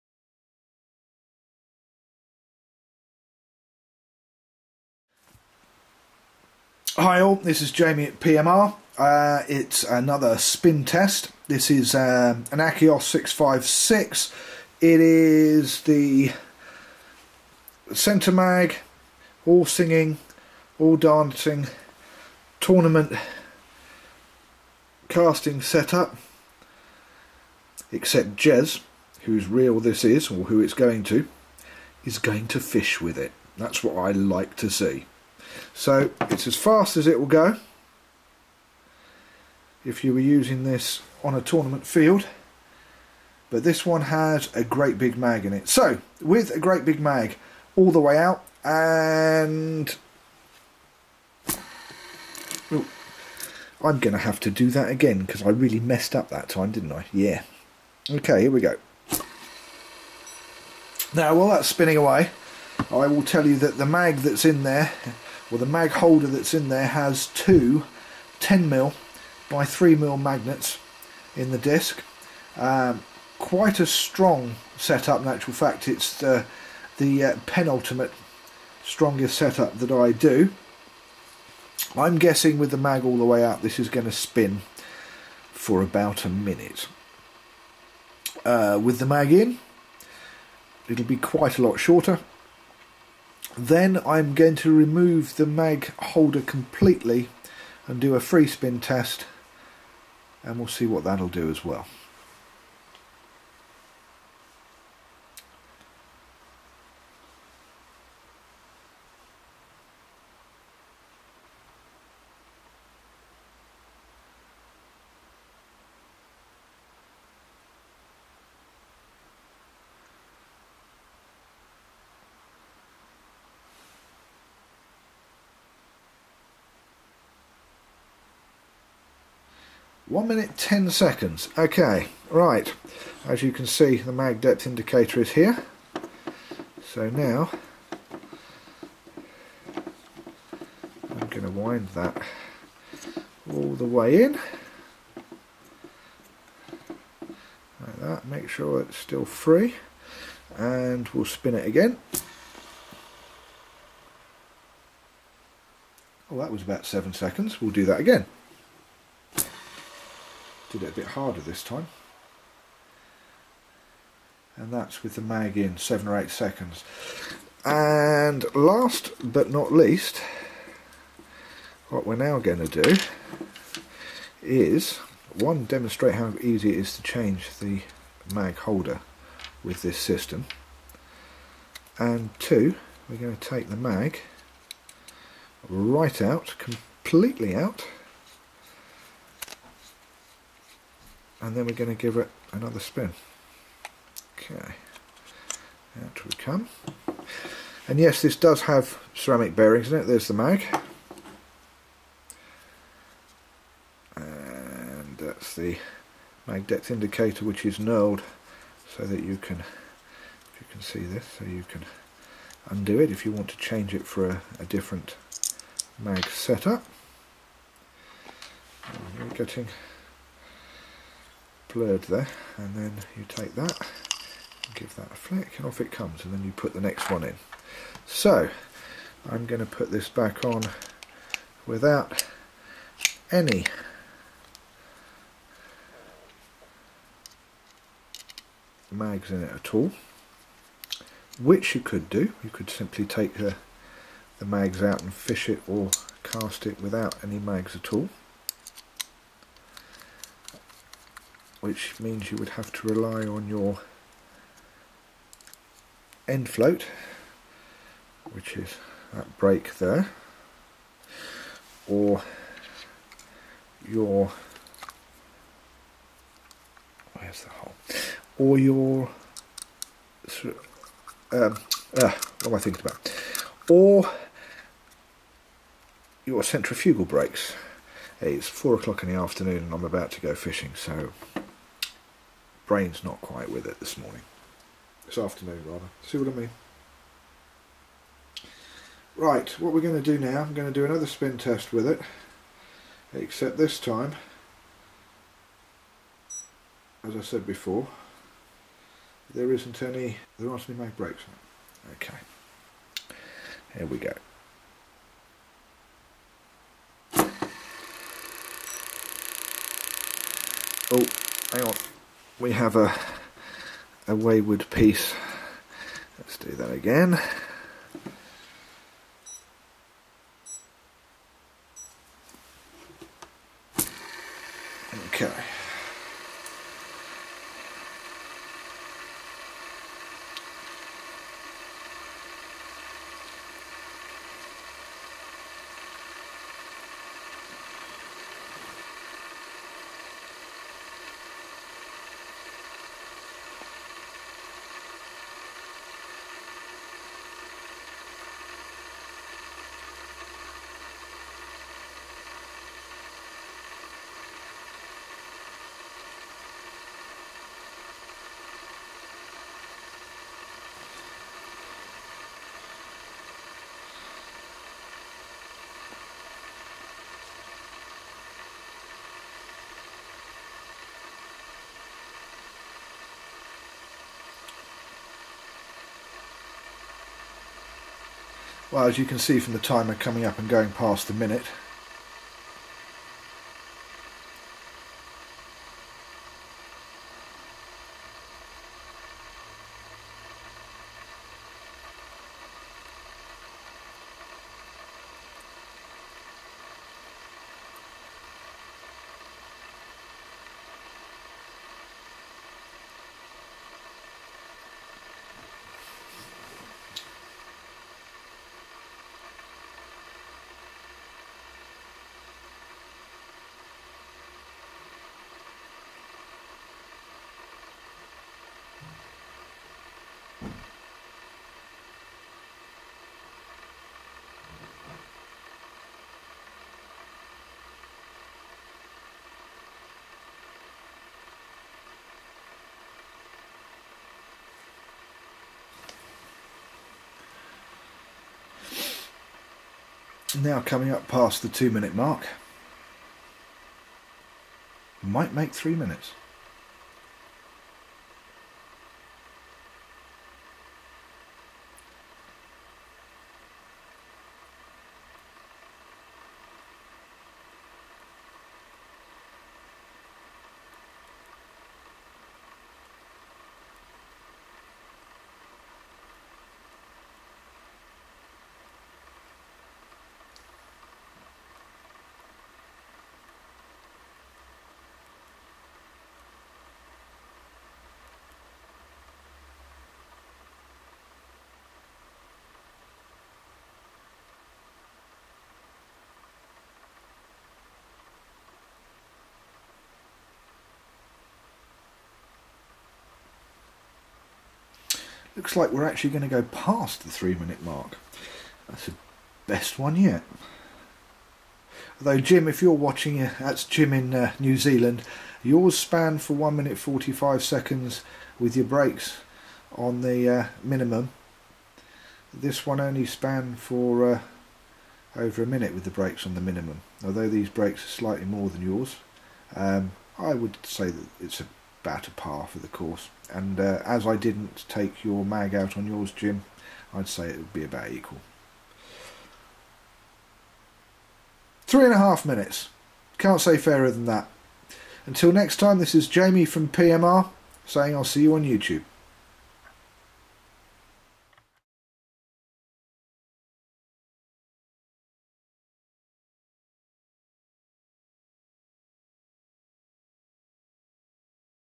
Akios PMR Bespoke Custom Tournament Super Tuned Conversion spin test, Jez reel
Akios 656 with full PMR Bespoke Custom Tournament Conversion. Spin test with mag brake out, spin test with mag brake in and spin test with mag brake removed.